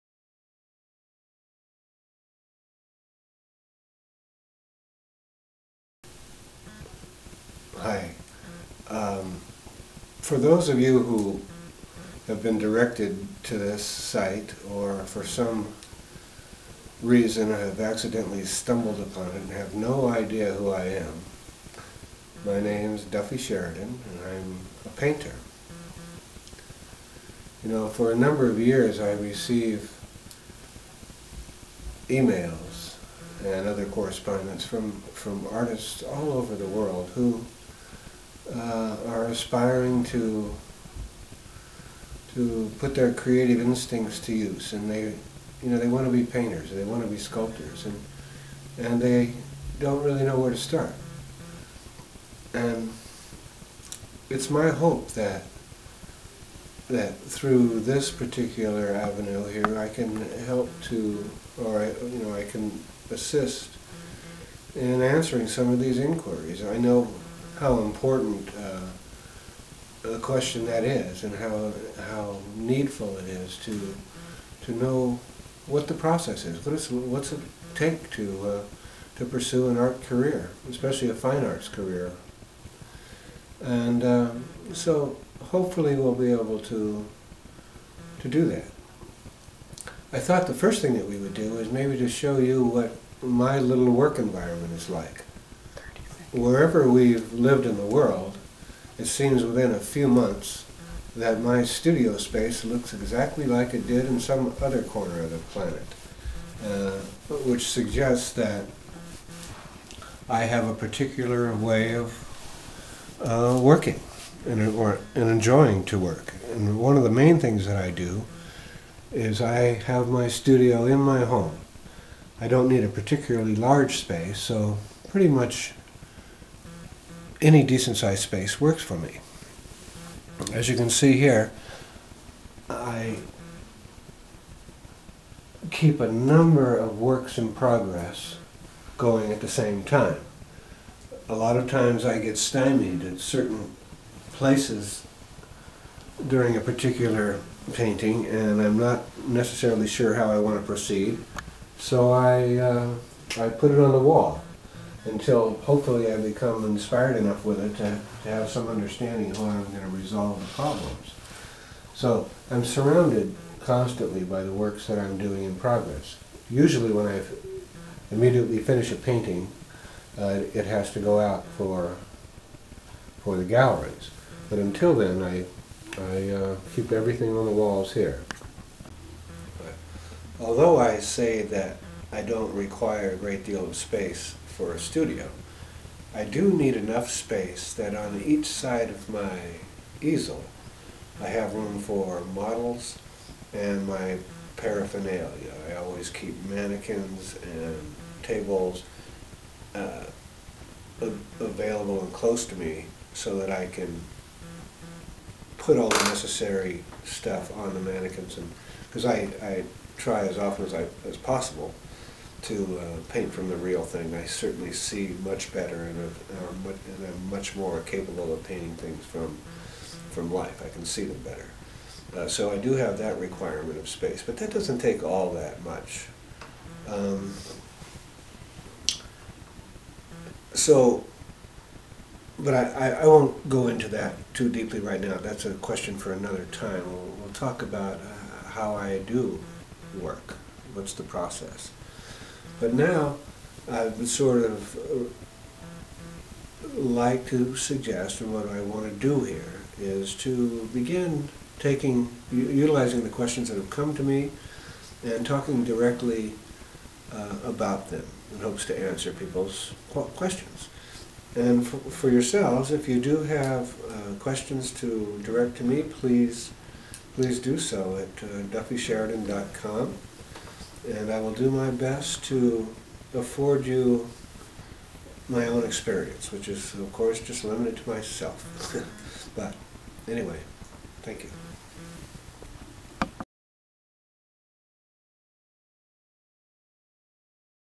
01 Intro & Tour
My first video, describing who I am, a tour of my studio, and what I plan to accomplish with a series of videos.